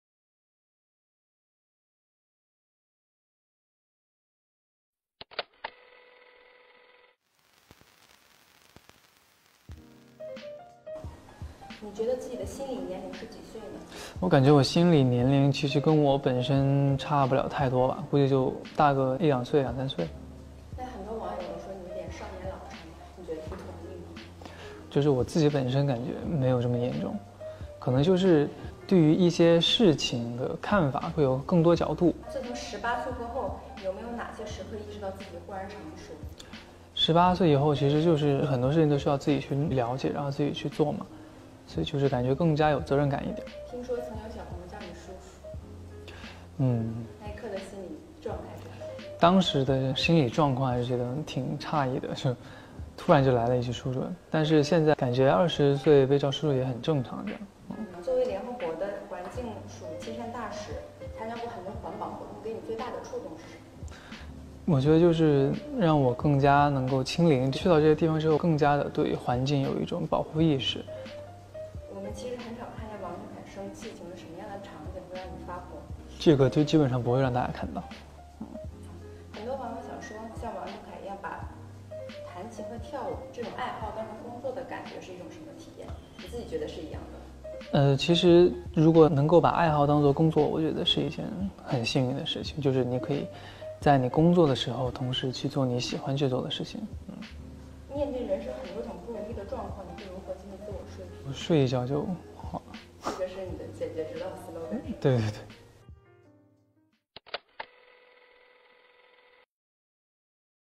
TFBOYS王俊凱 《嘉人NOW》年輕就真的是“後浪”們所擁有的樣子嗎？年輕真的是有年齡界限的嗎？
TFBOYS王俊凱 《嘉人NOW》年輕就真的是“後浪”們所擁有的樣子嗎？年輕真的是有年齡界限的嗎？這一切尚未可知。嘉人NOW，以真實的隨行視角記錄下了每一 位拍攝對象最真實的生活片段。分秒輪轉，讓我們回到當下的此時此刻。

我們和20歲的王俊凱，一起度過了難得又暢快的封面拍攝時間。一起聊天和分享：他認為自己的心理年齡是多少？面對“人生不如意”，會如何自我說服？

記錄下這些真實瞬間，這就是我們想展現給大家的-一些簡單的，真實的，就在身邊正發生著的，言之有物的意義。

視頻：@嘉人NOW